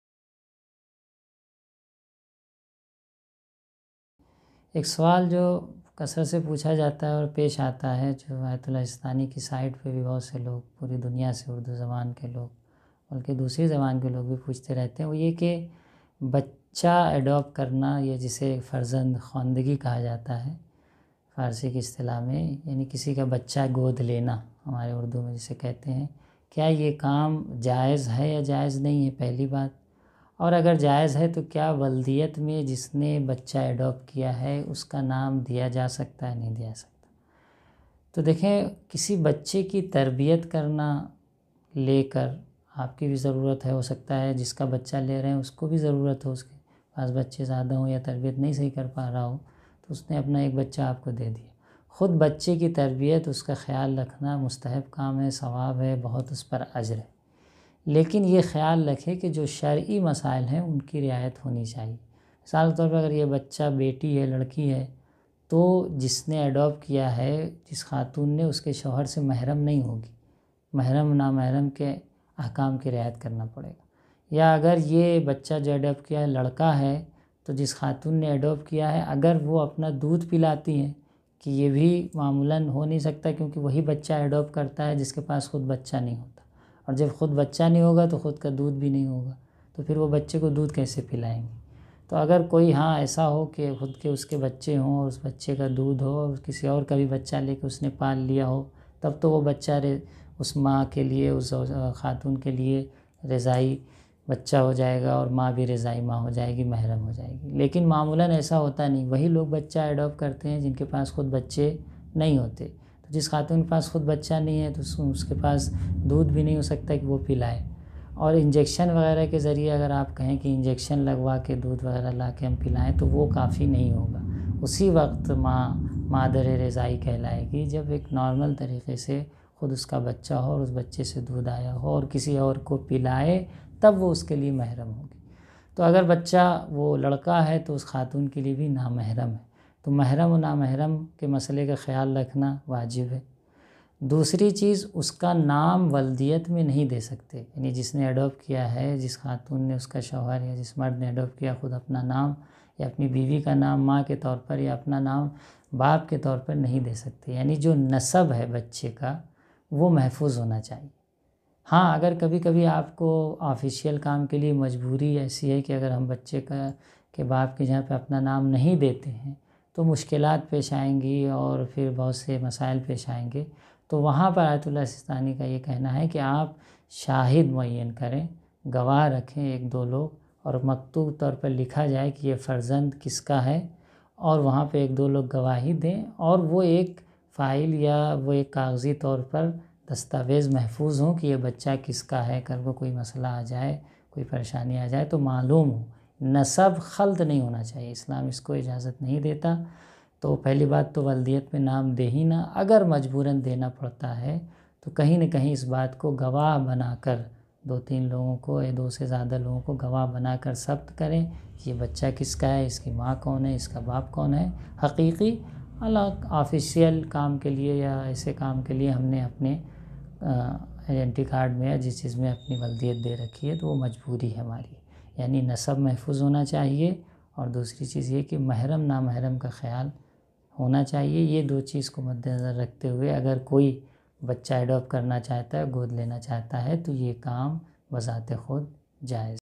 Kisi Ke Bachche ko God Lena (Baby Adop Karn) ... ? || Shar'ee Masael شرعی مسائل || Payam TV
Shar'ee Masael شرعی مسائل
       ::::::::::::::::: LIVE :::::::::::::::::
Kisi Ke Bachche ko God Lena (Baby Adop Karn) Jaez Hai.. ?

👉 Kisi Ke Bachche ko God Lena (Baby Adop Karn) ... ?
👉 Payam Tv
👉 Gest: Maulana S. Payam Haider Rizvi sb (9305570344)
👉 Production Year:  February 2021